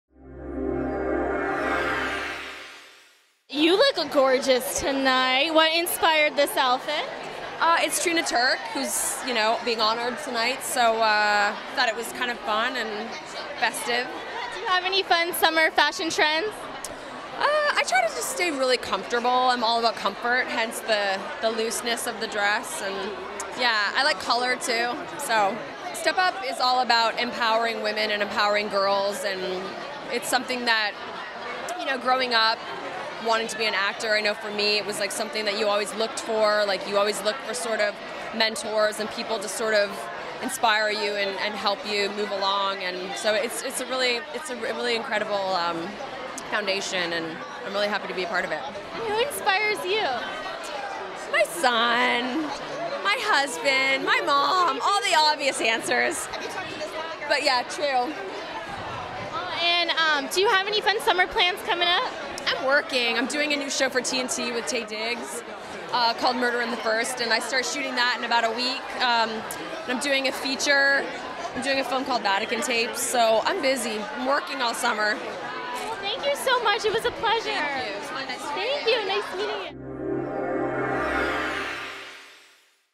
Kathleen Robertson Interview 2013 Inspiration Awards ARRIVALS - 90210 - Boss
Click to

Kathleen Robertson Interview at Step Up Women's Network 10th annual "Inspiration Awards" Red Carpet Arrivals at Beverly Hilton in Los Angeles, Ca USA May 31, 2013 - Red carpet interviews by  - Camera by Navjot Kaur